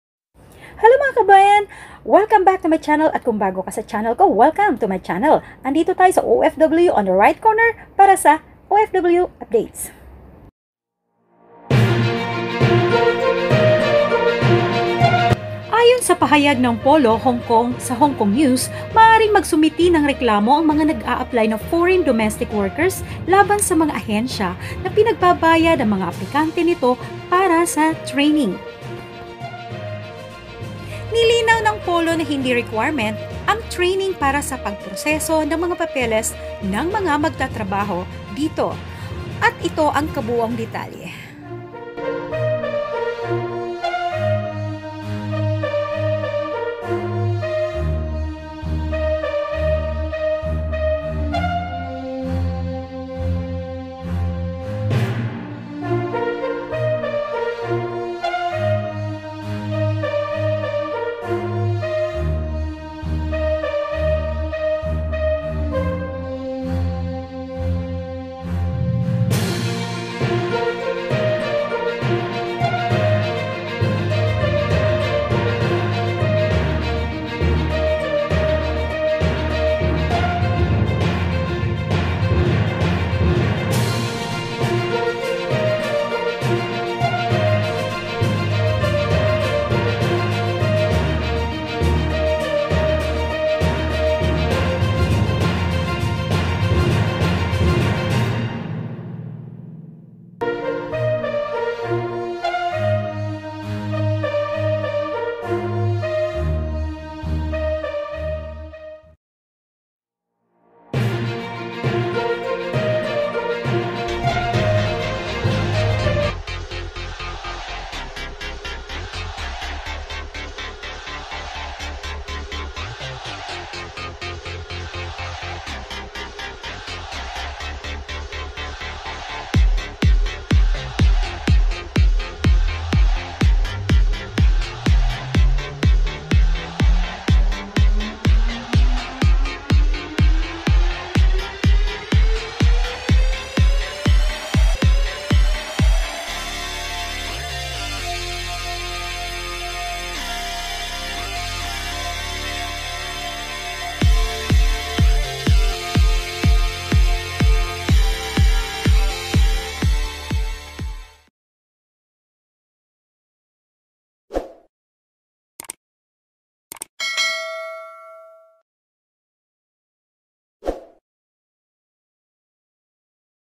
TRAINING NG MGA OFW HINDI KAILANGAN AYON SA POLO / OFW UPDATES / Maid in the Kitchen
Ayon sa PHILLIPINE OVERSEAS LABOR OFFICE, hindi kailangan magbayad ang isang OFW para sa training fee or hindi rin requirement ang training sa pag proseso para sa pag a apply ng trabaho sa ibang bansa tulad ng Hongkong.

Panuorin ang kabuoang detalye sa videong ito.